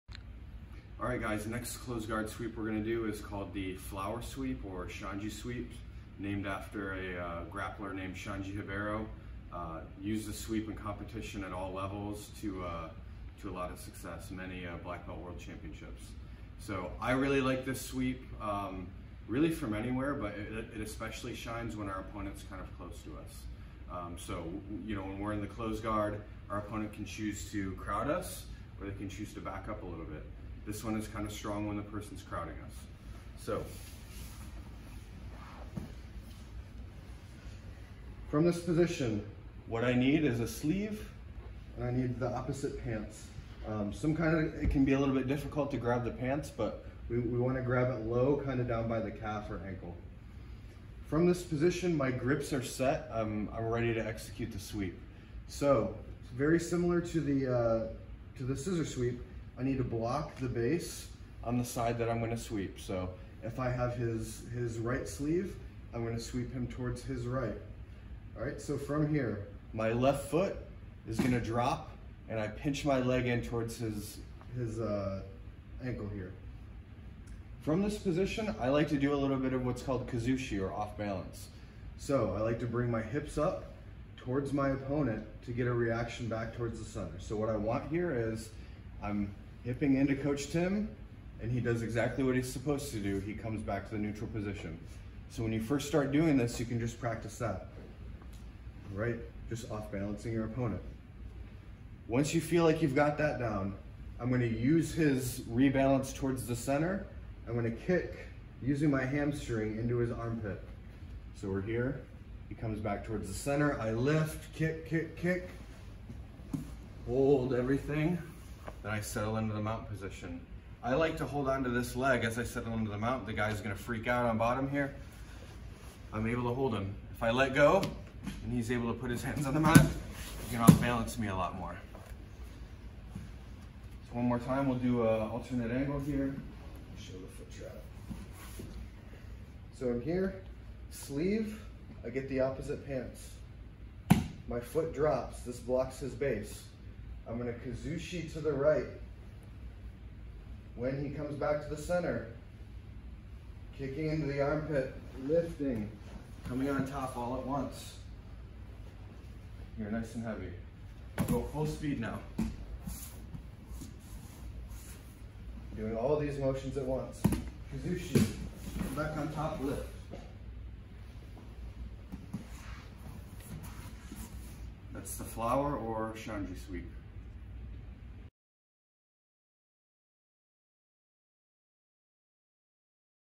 HNBJJ Basics - The Flower/Xande Sweep
High Noon BJJ's Coach Matt Miller walks through the legendary Xande (Flower) Sweep.